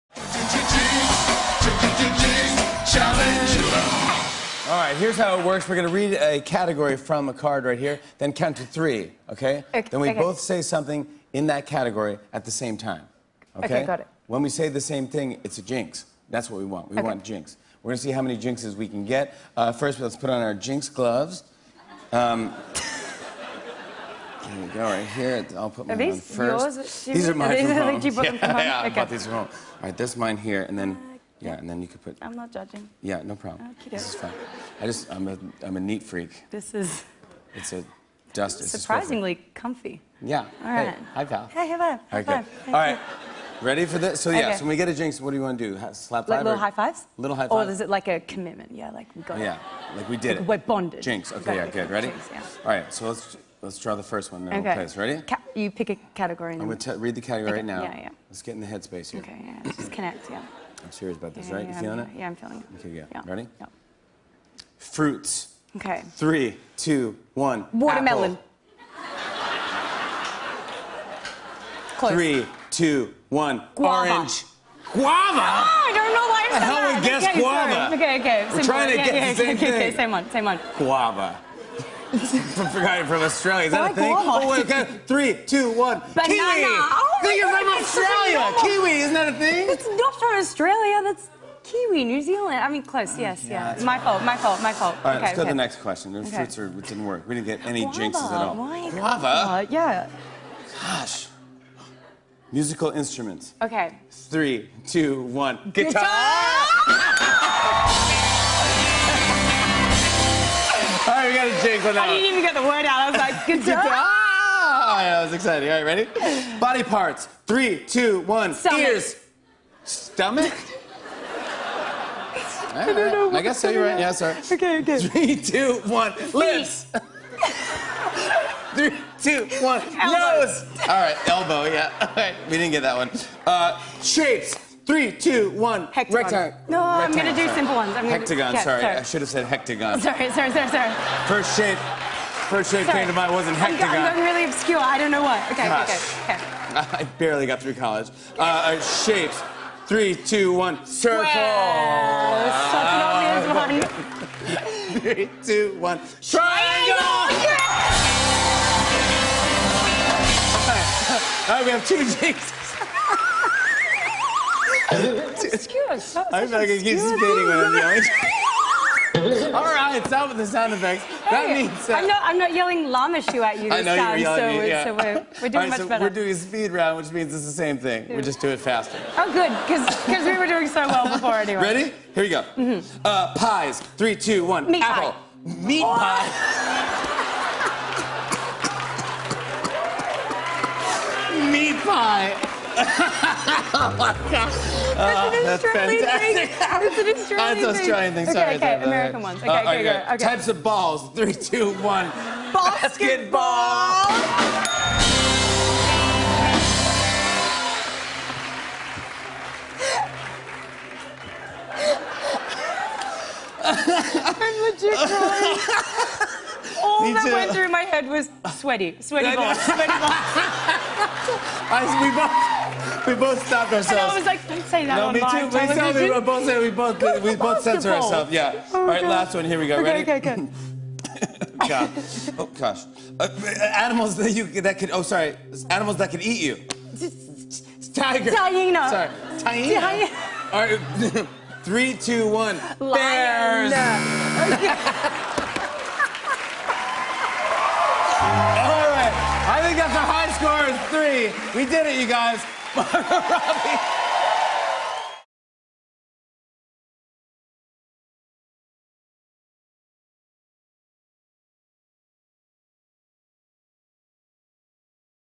Jinx Challenge with Margot Robbie
Jimmy and Margot draw random categories then try to shout out the same word at the same time to jinx each other.